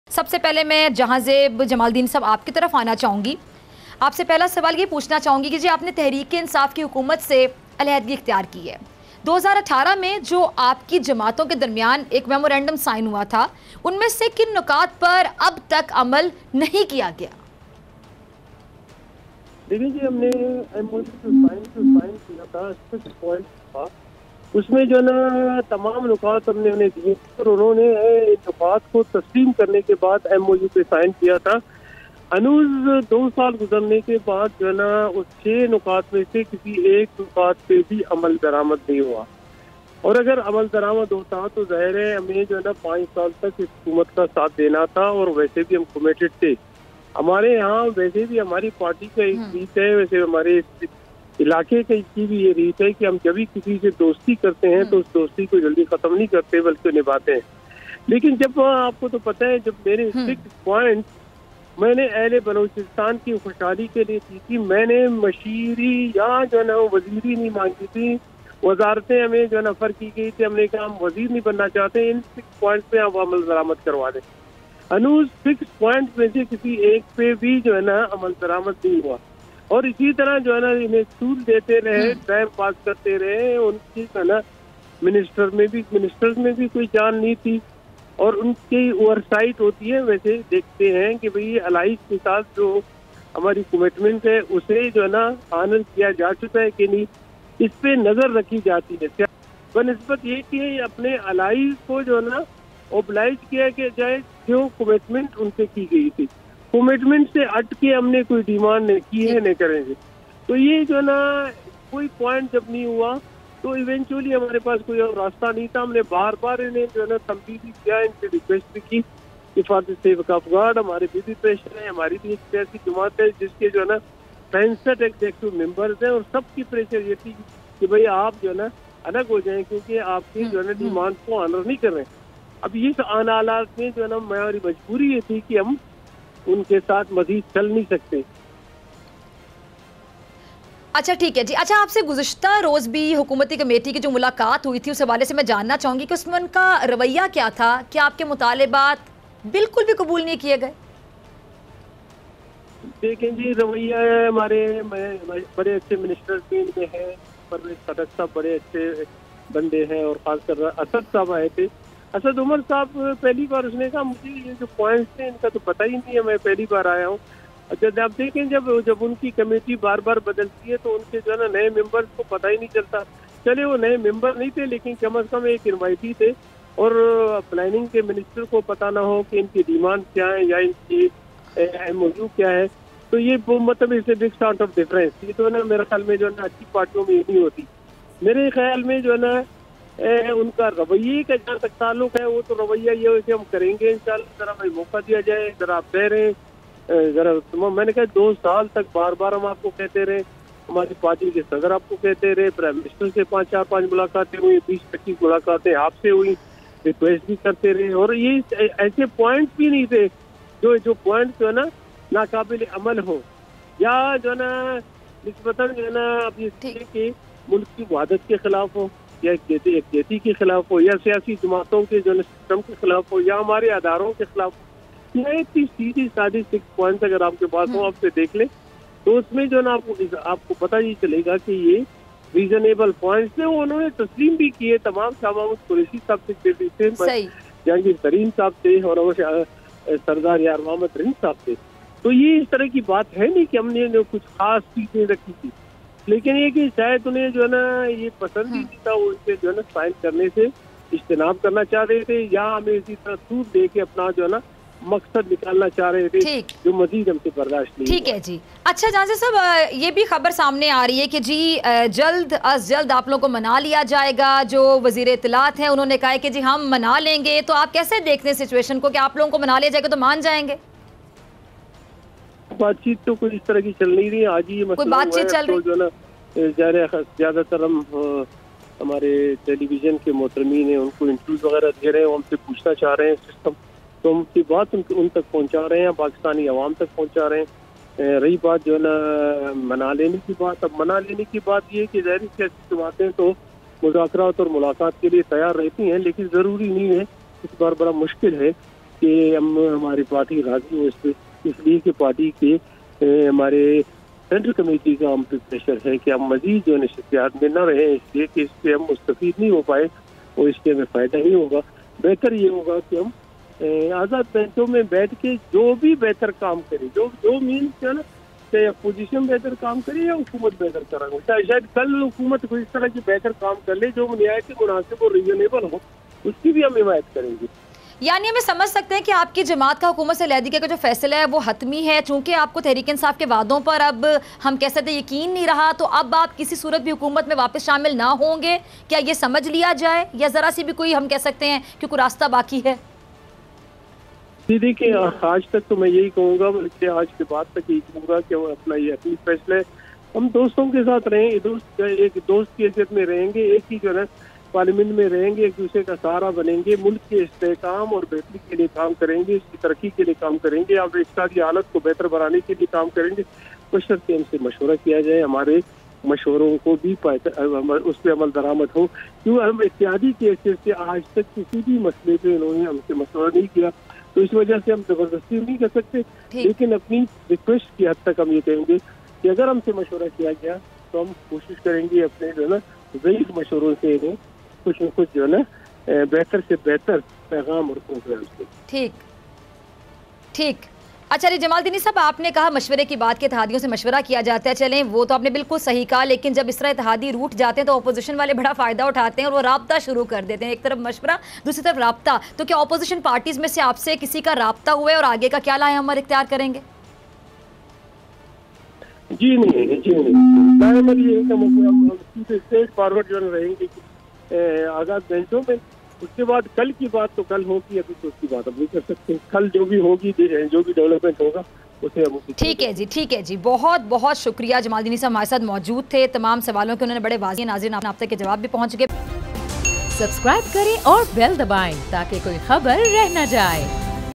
Senator Jehanzeb Jamaldini Interview with Fiza Akbar Khan, BNP Separated from PTI
BNP-M chief announces withdrawal from PTI coalition govt